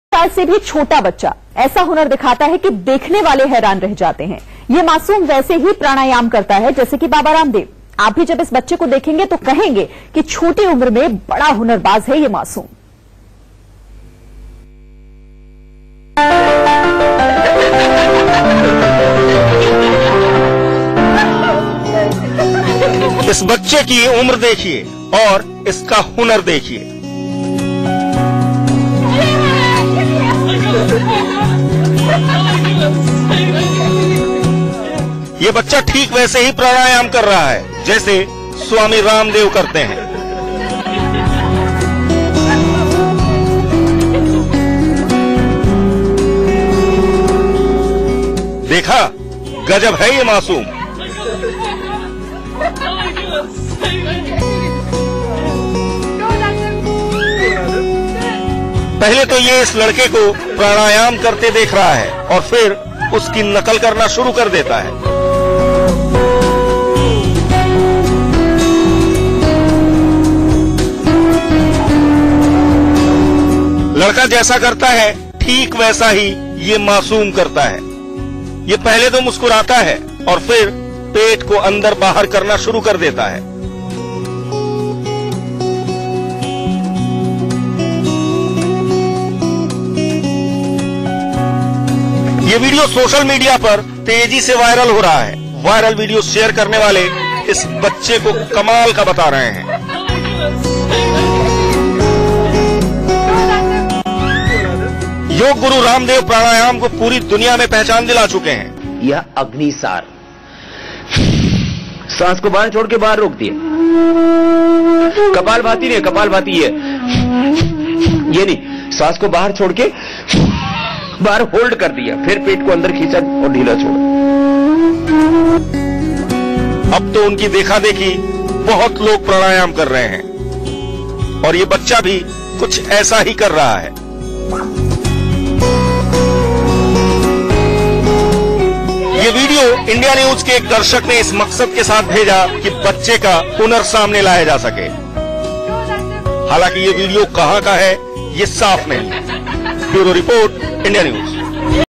उम्र छोटी, हुनर रामदेव जैसा ! एक साल से भी छोटा बच्चा प्राणायाम करता है
एक साल से भी छोटा बच्चा. ऐसा हुनर दिखाता है कि देखने वाले हैरान रह जाते हैं । ये मासूम वैसे ही प्राणायाम करता है जैसेकि रामदेव । आप भी जब इस बच्चे को देखेंगे, तो कहेंगे कि छोटी उम्र में बड़ा हुनरबाज है ये मासूम ।